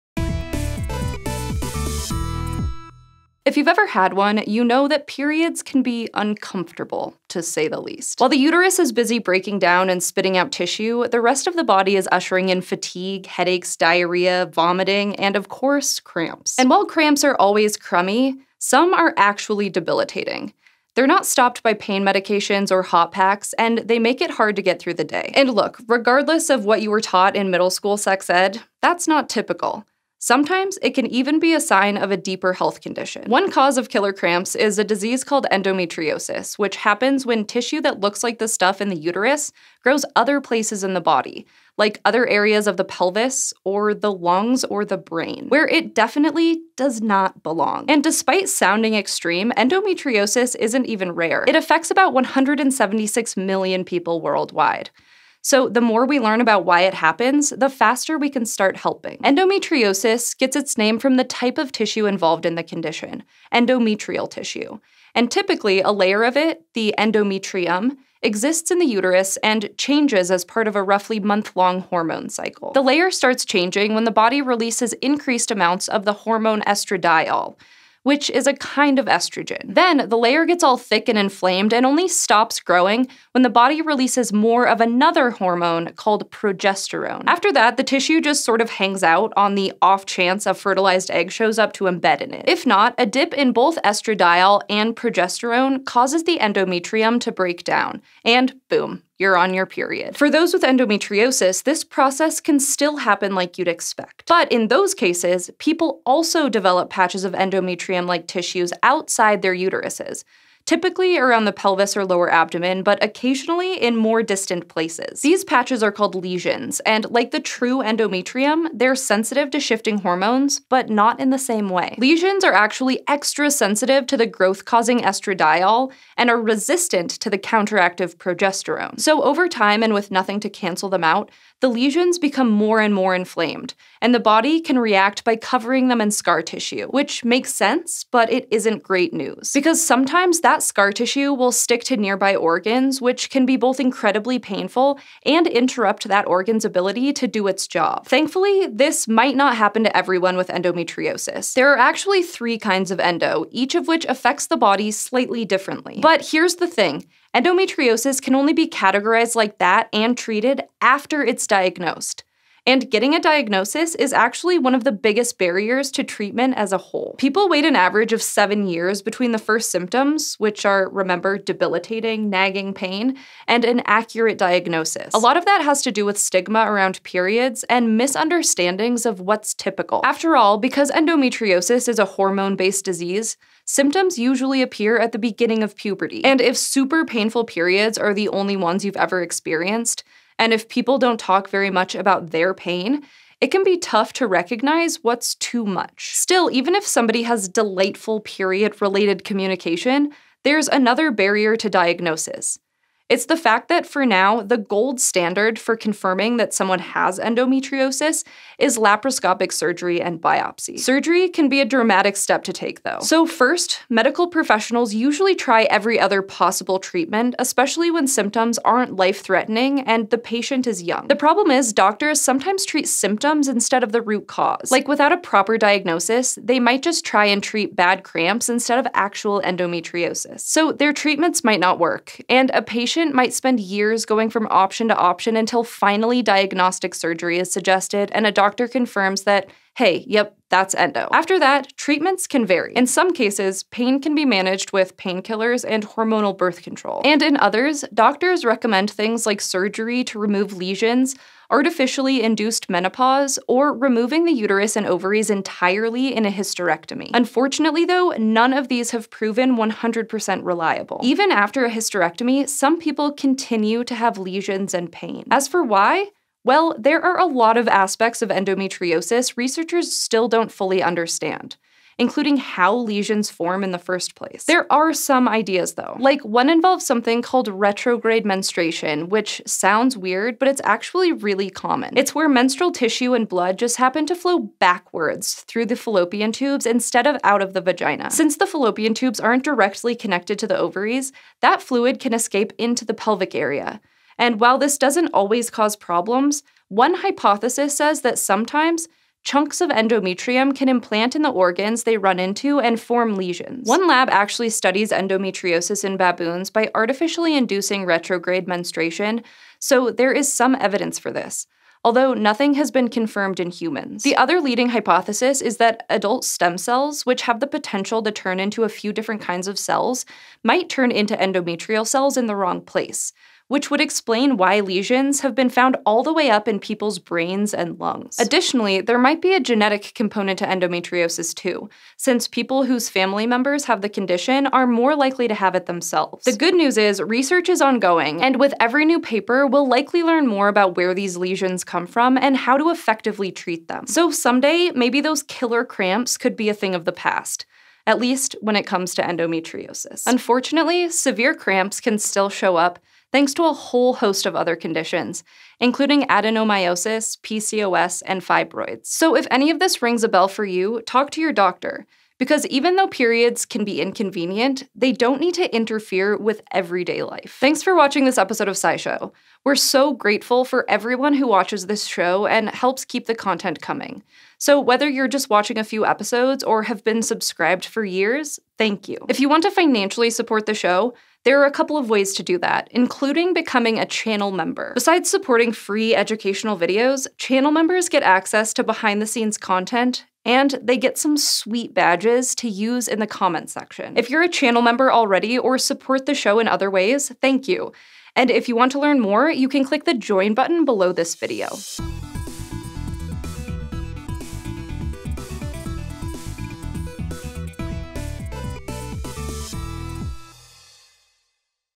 Endometriosis: When Uterine Tissue Goes Rogue
While period cramps are never fun, they can actually be debilitating for those with endometriosis - a disease where rogue uterine tissue turns up in places that it doesn’t belong. Like in other areas of the pelvis… or even in the lungs or the brain.

Sources:
(including speaking to the scientific director Professor Linda Griffith)